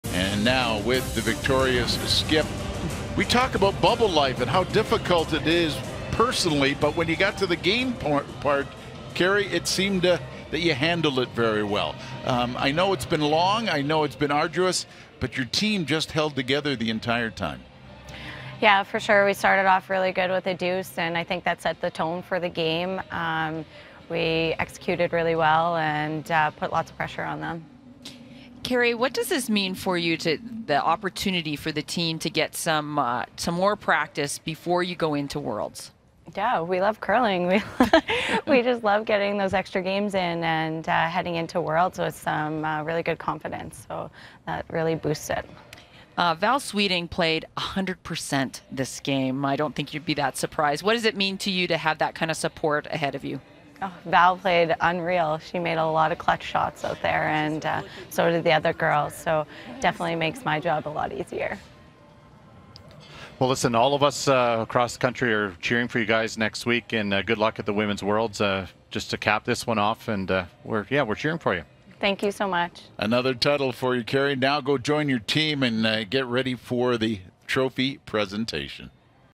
Kerri Einarson on winning the 2021 Princess Auto Players' Championship
Sportsnet's curling crew caught up with Kerri Einarson right after winning the  Princess Auto Players' Championship women's title.